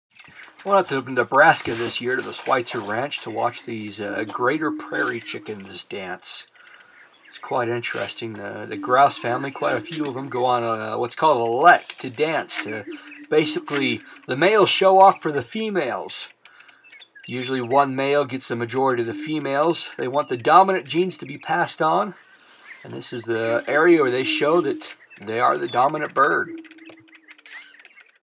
Greater Prairie Chicken 2016!
A spring visit to the Switzer Ranch in Nebraska. On the crisp mornings on the Switzer Ranch, visitors have the opportunity to sit in a bling and watch the prairie come to life.